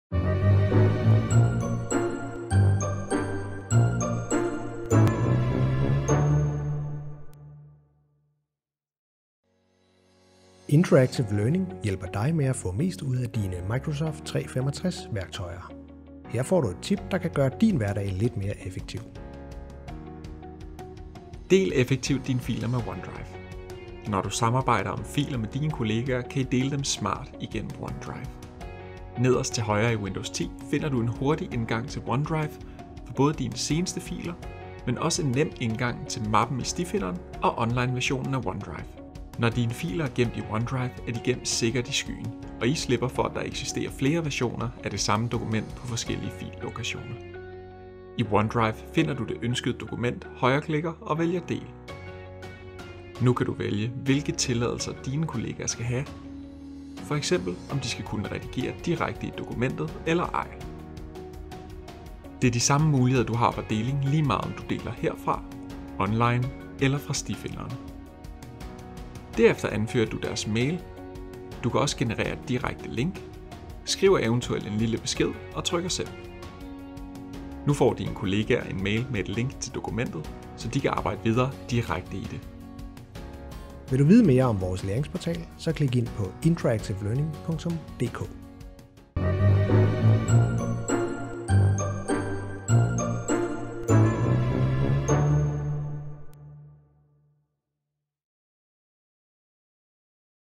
Låge nr. 17: Den 17. december
Del dine filer effektivt med OneDrive. Husk at deltage i vores konkurrence om én billet til en valgfri Masterclass i enten Office 365, Microsoft Teams, SharePoint Online, Power BI eller Power Apps